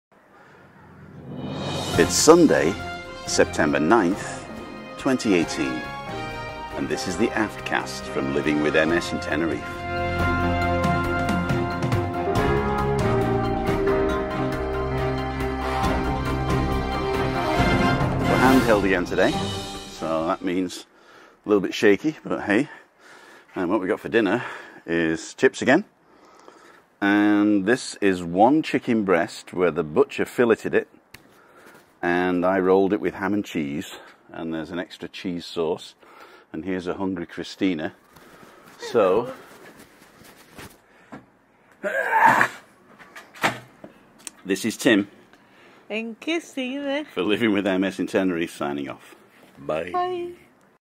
What was the weather like in TENERIFE SUR on Sunday, September 9, 2018...?
LWMST AFTCAST from Sunday, September 9, 2018... a VERY short one tonight... 27°C in the shade very hot in the sun... watch out you don't burn... Chicken cordon bleu rolitos for tea... G'Night Y'all...